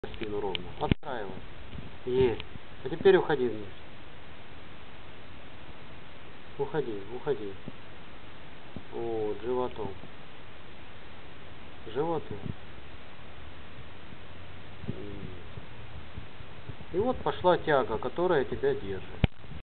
Тяга сдерживания в верхнегрудном отделе. Диагностика в наклоне. IMGP0868
Представлен способ определения блокировки верхнегрудного отдела позвоночника в наклоне вперед в положении сидя.